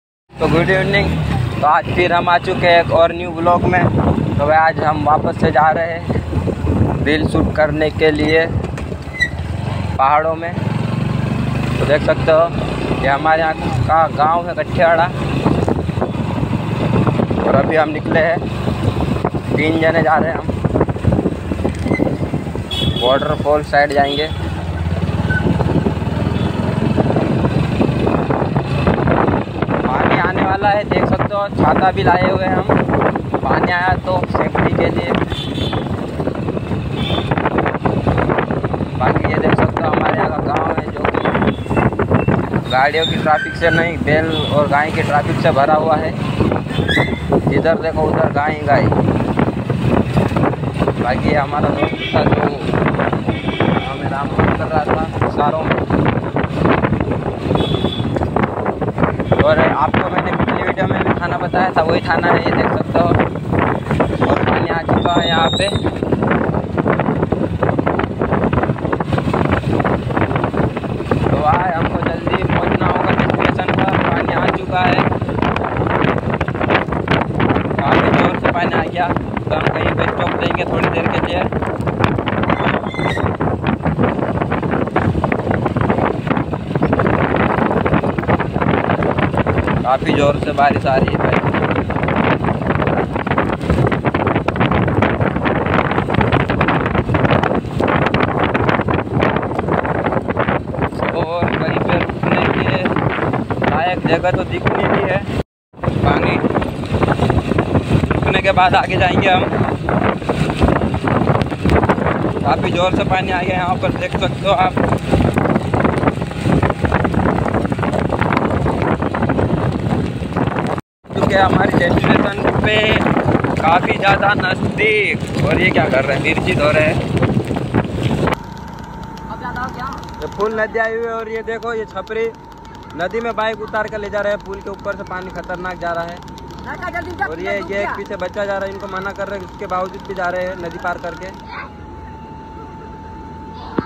PUL KE UPAR SE KHATRNAK PANI JARA OR BACHA KUD GYA😱😱@TTmp69
Hello sabhi bhaiyo ko🙏